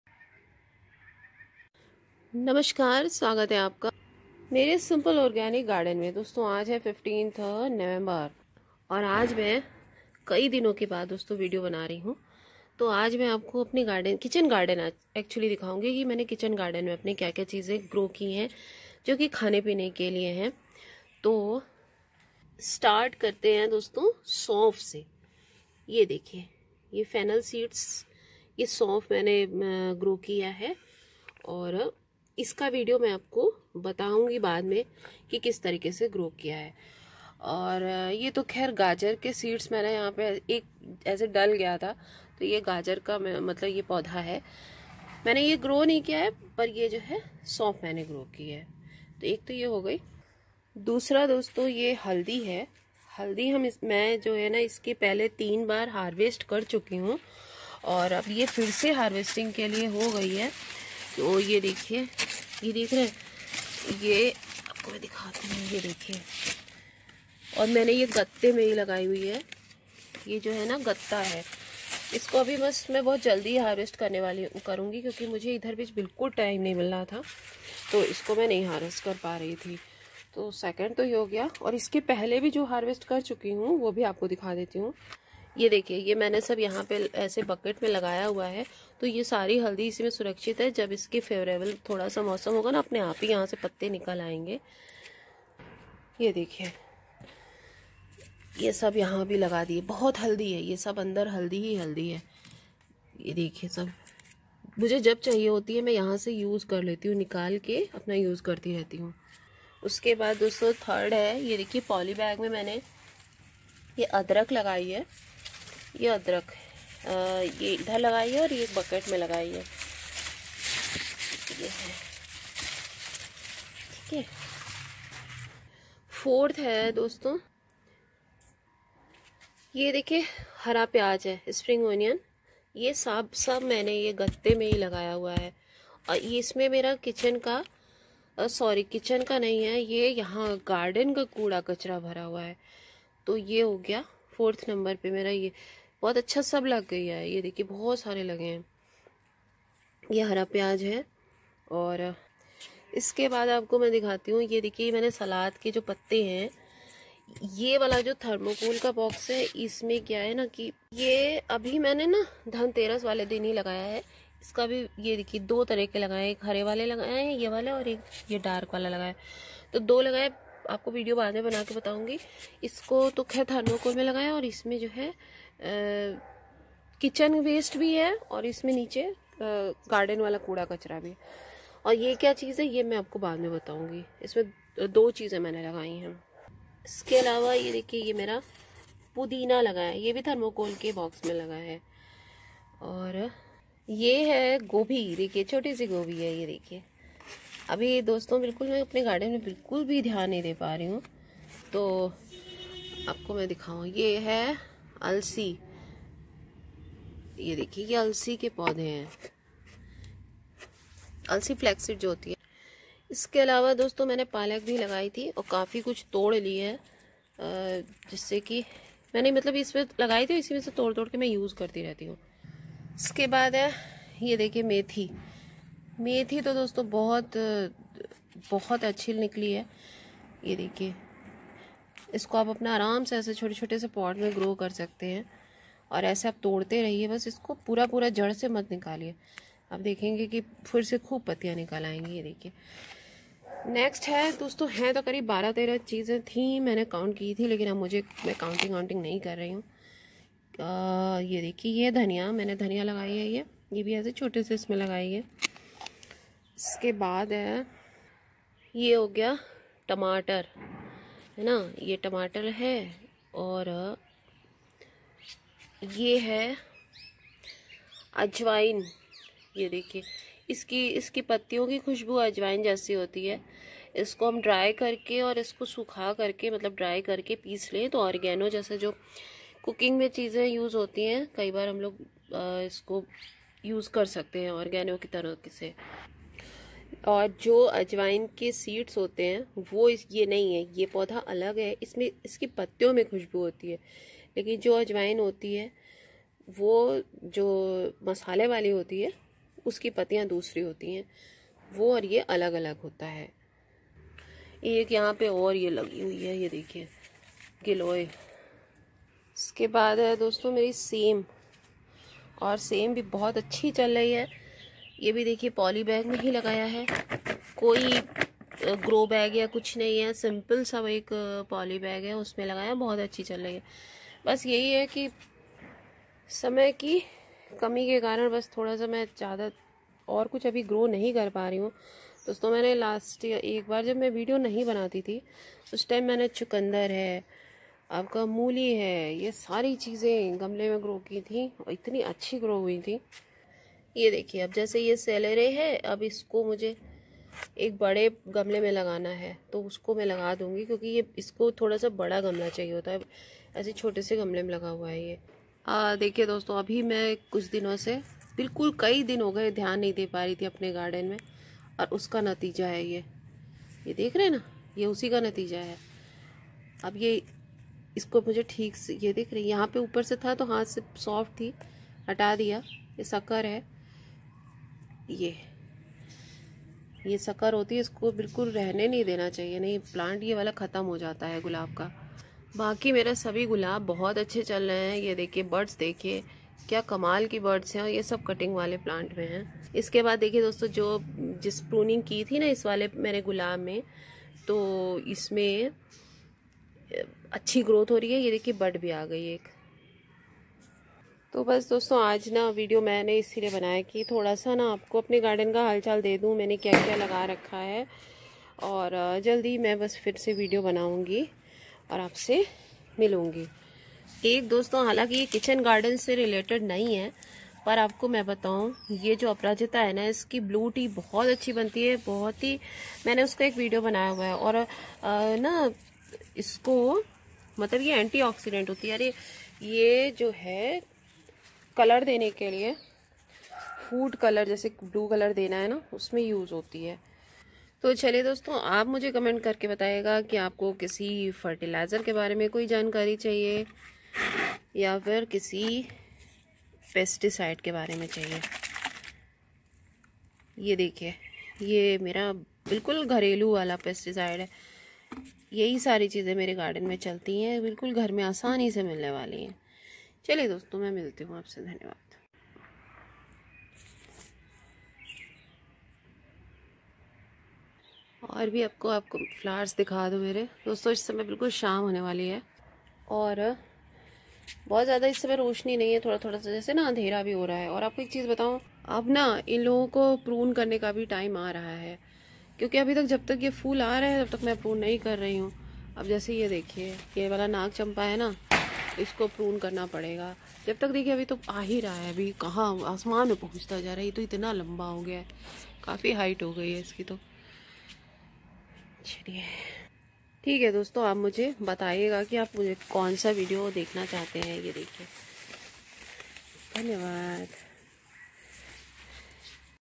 Mera Kitchen_Garden Plants Vegetables Spices in Waste Material Pots Aap bhi Grow Kar sakte ha
How to grow Methi Fenugreek Seeds in pot

Rose Pruning

Polybags Cardboard Box Thermocol Box Waste Material

My Kitchen Garden Overview Update
1#Turmeric / Haldi
2#Fennel Saunf
3#Methi Fenugreek
4#Tomato
5#Mint
6#Coriander
7#Ajwain
8#SpringOnion
9#Lettuce 🥬
10#Sem/
11#Ginger
12#Celery
13#Spinach